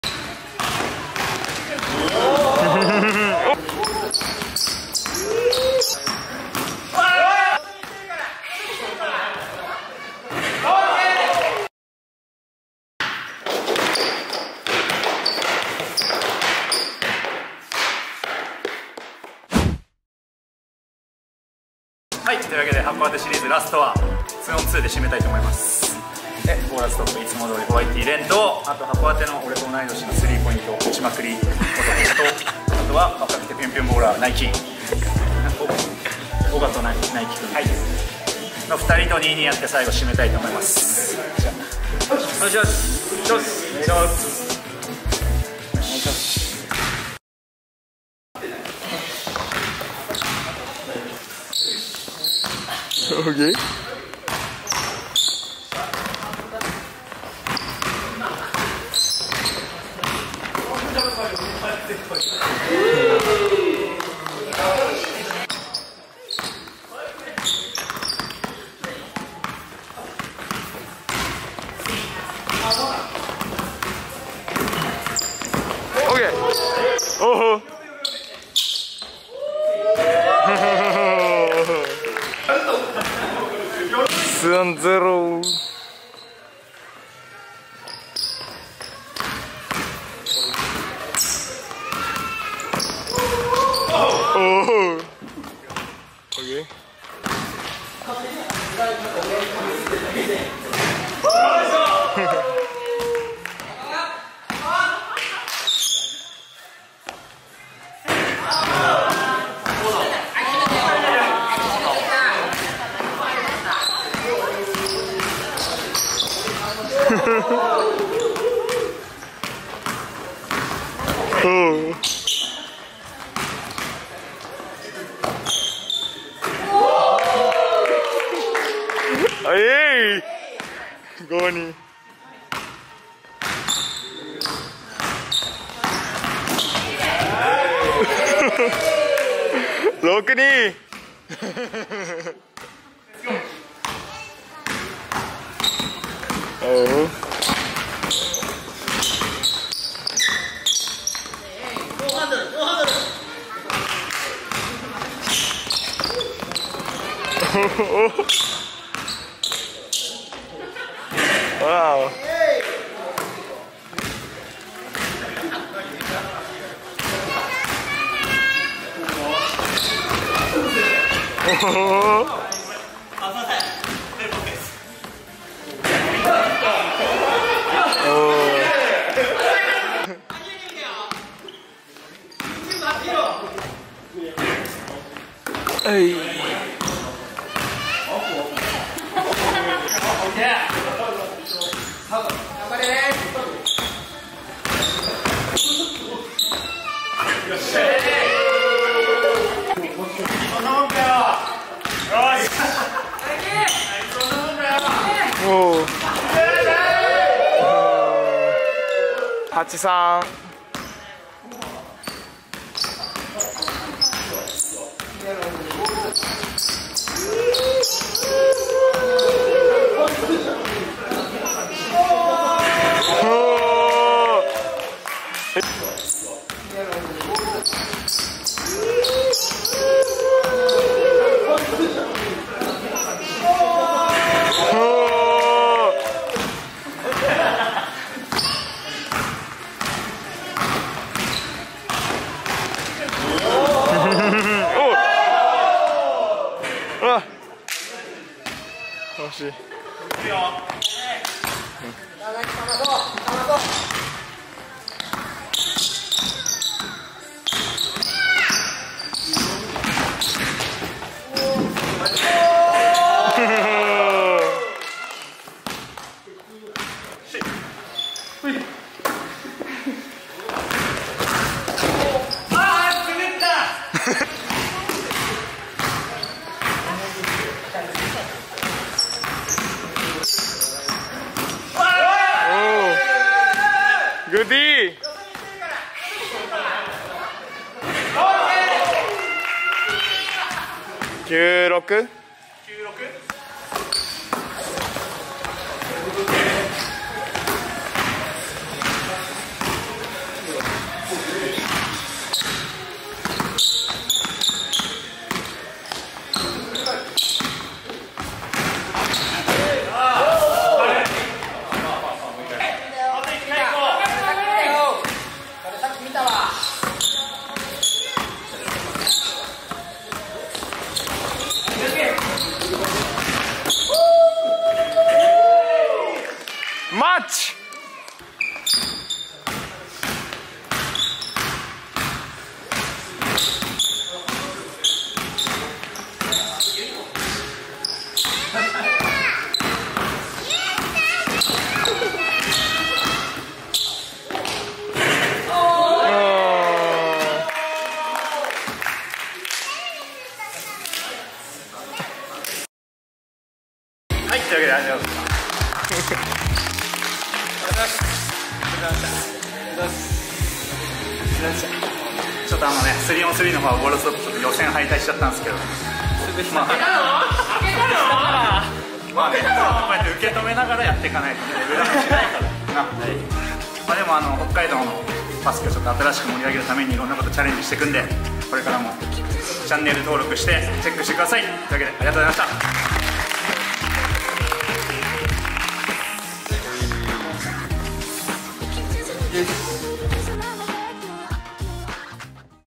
【バスケ】チームハンドル vs 3P打ちまくりシューター & キレキレヤングボーラー
函館ゲームラストは2on2で締め⛹️‍♂️
ballerztop whitea & Ren によるチームハンドルが
3P打ちまくりシューターOGAとキレキレなヤングボーラー naikiによるチームHAKODATEと対決🔥